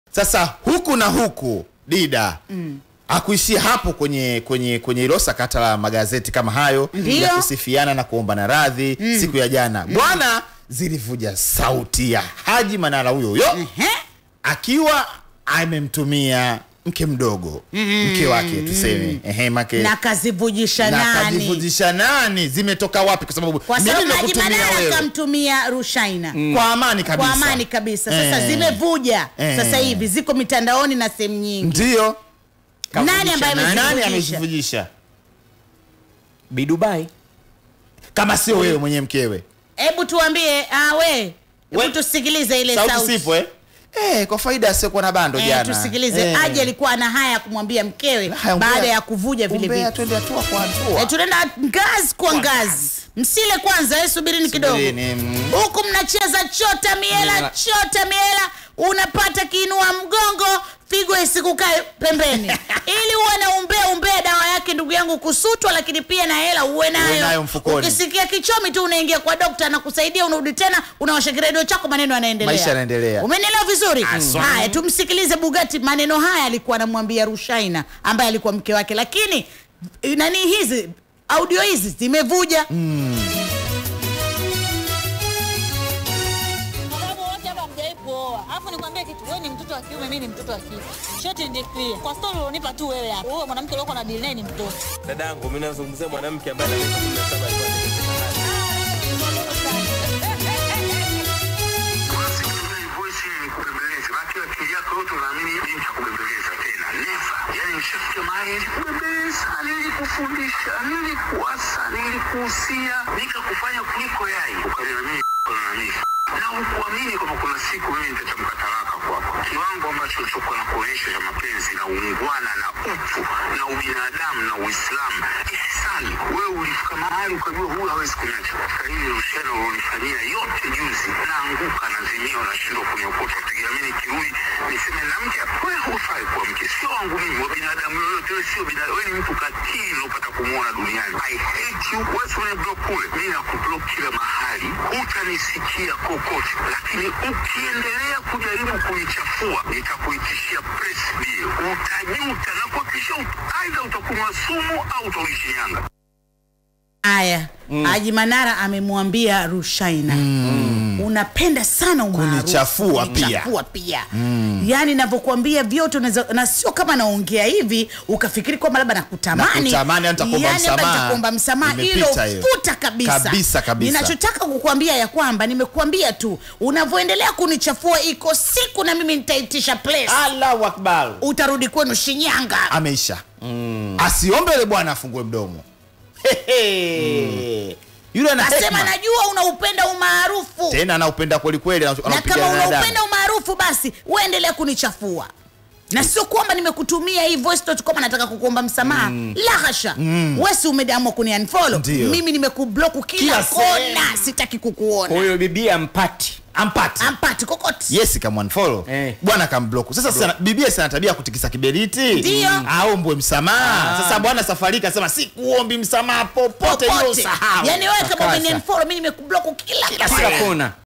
HAJI MANARA AMPA MANENO MKE MDOGO - ''SIWEZI KUKUOMBA MSAMAHA KAMWE, WEWE UMENI-BLOCK ENDELEA"
88.9 DAR/ZNZ/PWANI 📻 | 104.1 DODOMA 📻 | 94.5 ARUSHA 📻 | 97.3 MBEYA 📻 | 94.9 MWANZA 📻 | SHINYANGA 106.3 📻 | KIGOMA 101.7 📻